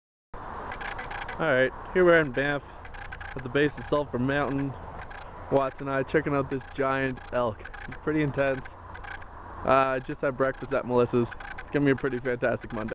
Giant Bull Elk - Banff Alberta
Walking through the woods I stumbled upon this stud..